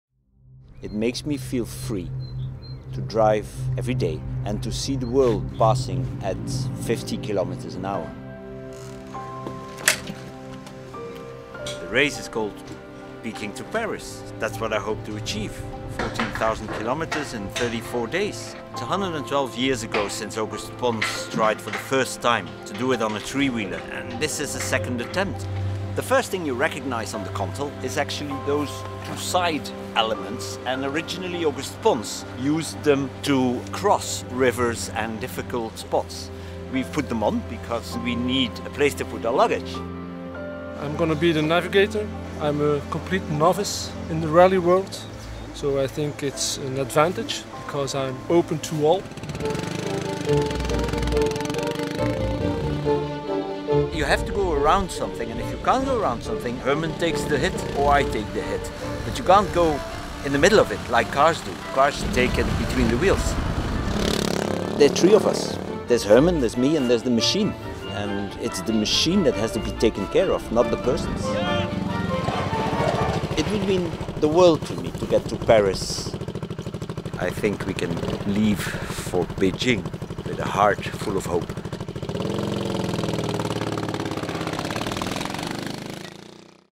Ghost of lost 1907 Peking Paris 3 wheel Contal back to life - Short Video
Mototri Contal jolted into life at Dutch test track ready for second P2P (2019) assault 112 years later.

 The first Peking Paris in 1907 comprised five competitors, just one of them didn’t make it to Paris 112 years ago, a French trike. Now Belgian architect Anton Gonnissen has revived a 1906 Contal, determined to complete the event Auguste Pons failed to finish when his three wheeled motorcycle ran out of fuel in the Gobi desert. The original Contal is still there, somewhere.

After a long and sympathetic rebuild, involving one of the world’s leading experts on antique cycle cars, the legendary Contal Mototri three-wheeler, was finally taken from the workshop to the test track, for a thorough shakedown, in late February 2019.

A full day had been given over for the trial at the EuroCircuit in Valkenswaard, the venue which has previously hosted Dakar machines and Rallycross events. Over the course of the session, the crew comprising Anton Gonnissen (58) from Sint-Martens-Latem and novice navigator Herman Gelan (46), an interior architect from Ghent, drove the machine hard. They tested across a selection of surfaces including rough and loose gravel, wet sand and sticky mud then smooth tarmac.

The idea was to recreate in one day, authentic representations of the sort of terrain likely to be encountered along the route of the Peking to Paris Motor Challenge. Be that the rugged wilderness of the Gobi Desert, the potholed Siberian lorry routes or a well surfaced European highway.

To add an extra dose of reality, the Contal was also fully loaded with the equipment which the crew would be carrying with them during their incredible journey to see how this extra weight affected the balance and cornering of the vehicle.

As soon as the trike had been rolled from its trailer, it started first time and throughout the day, with Anton and Herman on board, it lapped the track relentlessly. Their task was to expose any weakness which could to be rectified while there was still a workshop at hand. There was some hard braking, plenty of sharp turns and some rapid acceleration. The overall fuel consumption was closely monitored and the engine fluid levels checked at regular intervals.

This is no racing machine however, so Anton was respectful of its capabilities. “There will be three of us on this rally, there’s Herman, there’s me and there’s the machine. And, it’s the machine that we have to take care of if we want it to take care of us - and deliver us safely”.

With a little input from the crew therefore, some tweaks to the steering were made during the lunch break and there was some debate about the best tyre pressures to use but, overall the maiden voyage was an ‘out of the box’ success. In truth it had to be however as a mere three days later the Contal was to be packed away into a shipping container for the long sea journey to Beijing.

Sitting in the pits at the end of this long hard day, Anton, the driver and driving force behind the project declared himself “absolutely delighted” with his machine.

His softly sprung Harley Davidson saddle, the only form of suspension for the driver was pronounced a total success whilst his brave and unflappable - and first time navigator - Herman Gelan, noted that his position on the so called ‘suicide seat’, with a trip meter and GPS close to hand, was the ideal platform for him to keep them on the right course and “was nowhere near as bad as it looked”.

After just one day of riding it though, does the thought of spending 36 days aboard the Contal worry either of them? “Not in the least” they retort in unison. “It’s proved to be comfortable, capable and having seen what it can do, we’re even more convinced that we’ve built something which will deliver us to Paris”.

Anton, a Peking to Paris veteran already, went further and offered that “it will mean the world to me to get to Paris. If I don’t, I will have failed in my mission to honour the memory of Auguste Pons and we will go down in history as the second team not have made it to the finish aboard a Contal. We most definitely want to be the first”.